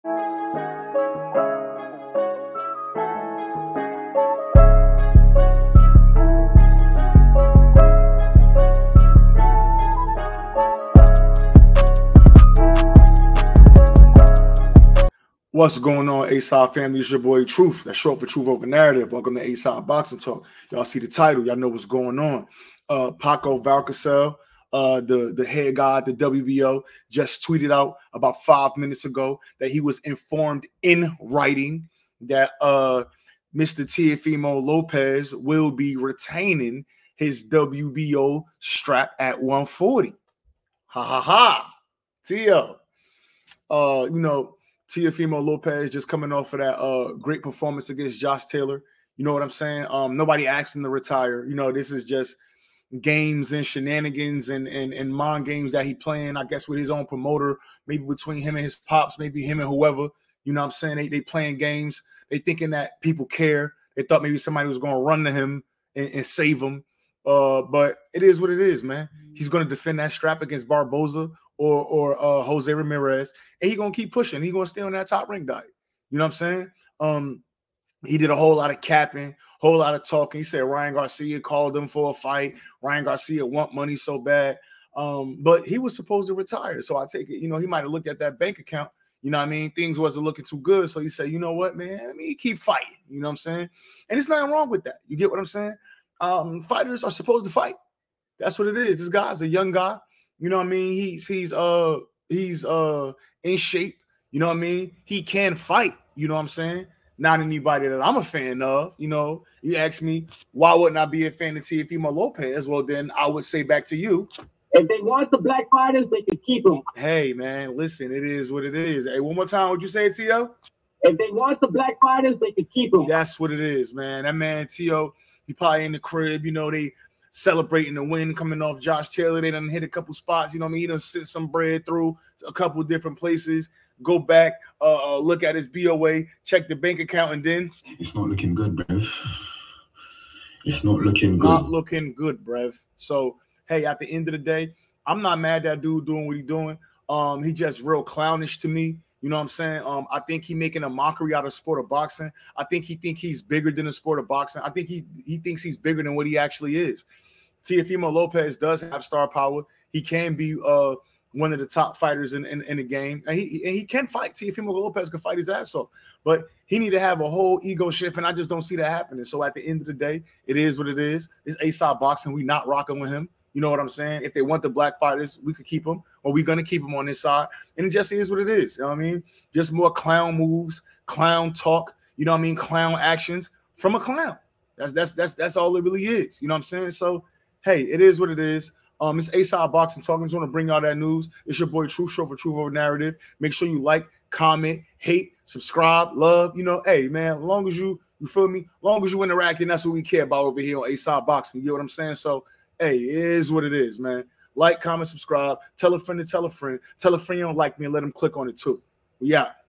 NOT BREAKING NEWS: TEOFIMO LOPEZ DOES WHAT WE..
Teofimo Lopez does what we all knew he would and retains his WBO title at 140. More clown work from him and his team.